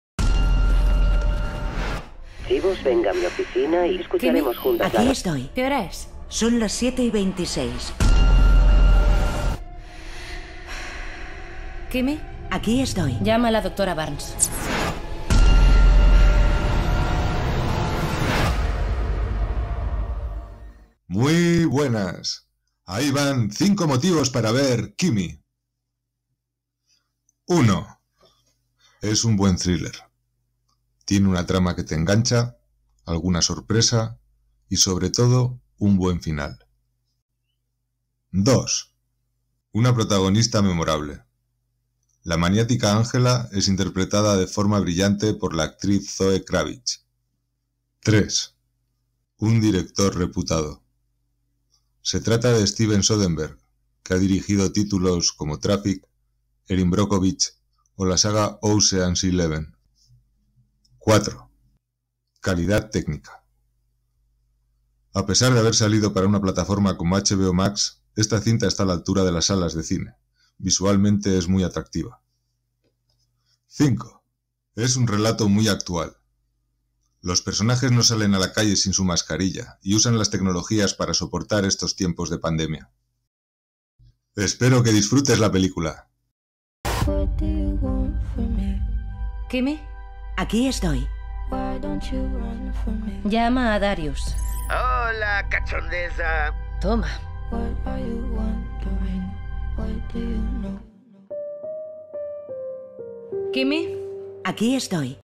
PELÍCULA RECOMENDADA: KIMI
Recomendaciones de cine sin spoilers, sólo novedades y recientes, sólo películas buenas.
KIMI (2022)
Disponible en: HBOmax
Género: Suspense
Dirección: Steven Soderbergh
Reparto: Zöe Kravitz, Erika Christensen, Jaime Camil